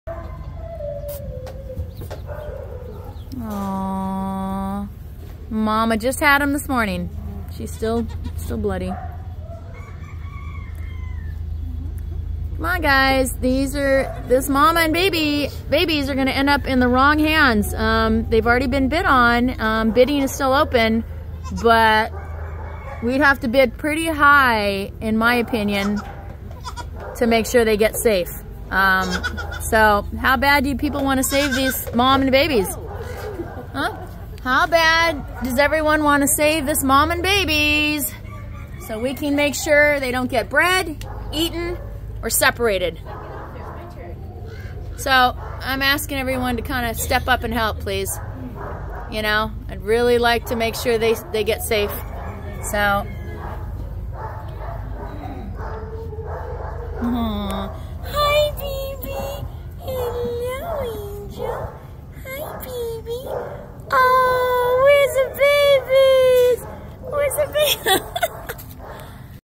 Mama gave birth at a kill shelter
Devore shelter,,, we are trying to save this family!!!